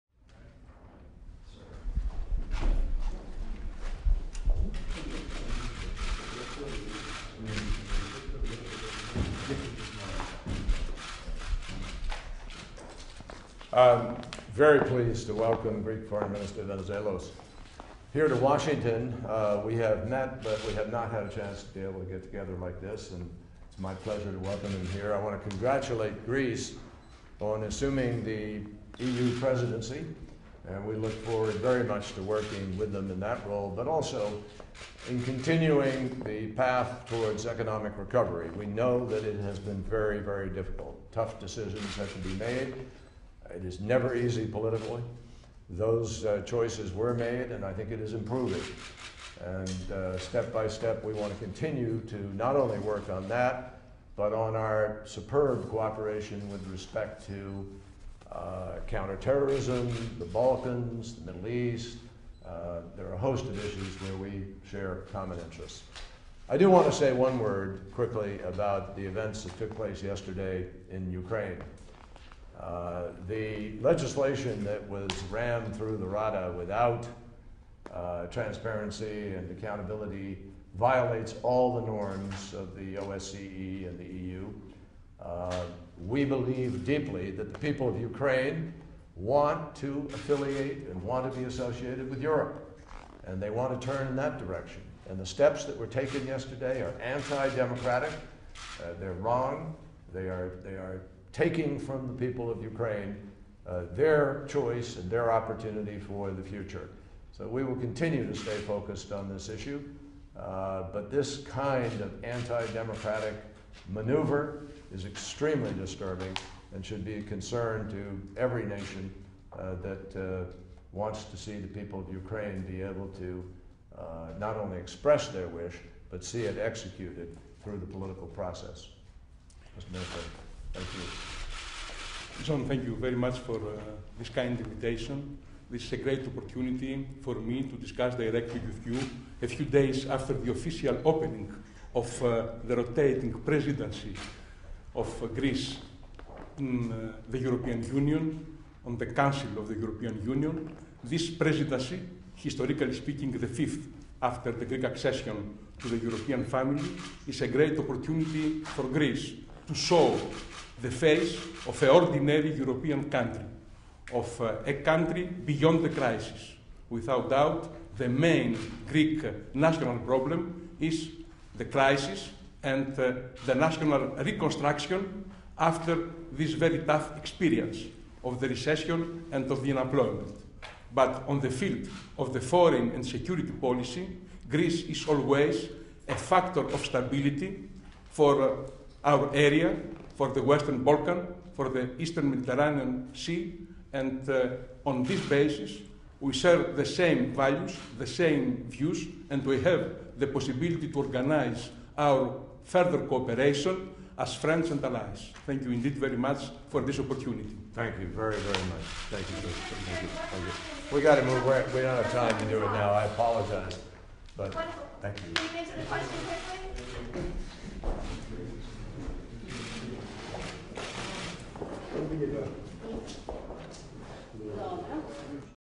Secretary Kerry Delivers Remarks With Greek Foreign Minister Venizelos
U.S. Secretary of State John Kerry delivers remarks with Greek Foreign Minister Evangelos Venizelos at the U.S. Department of State in Washington, DC on January 17, 2014.